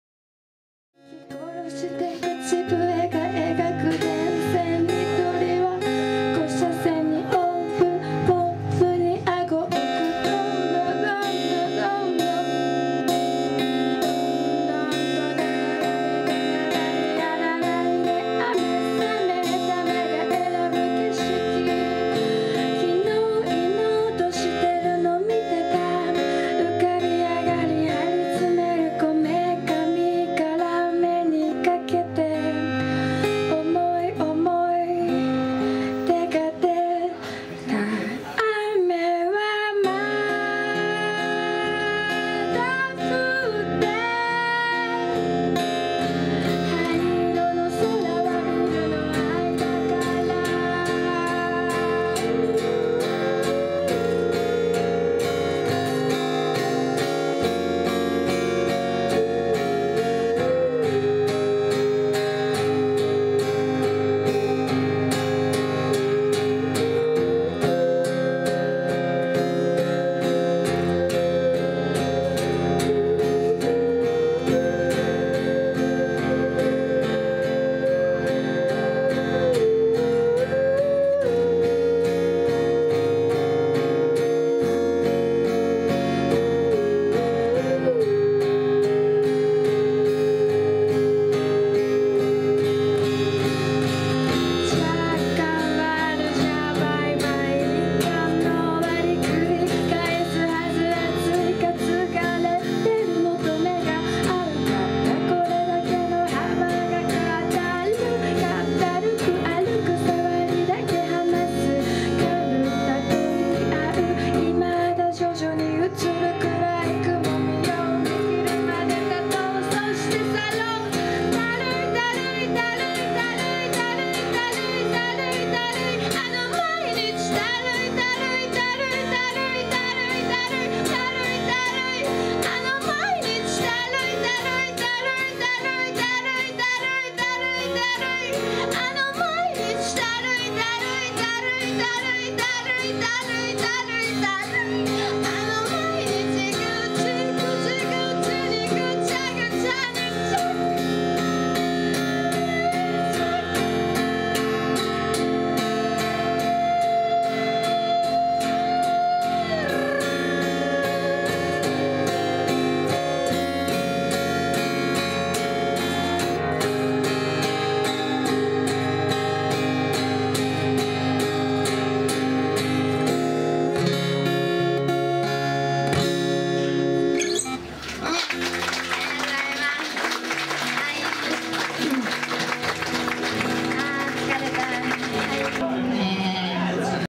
Nikaidoh Kazumi Live
Nikaidoh Kazumi shows of her vocal talent live in the US.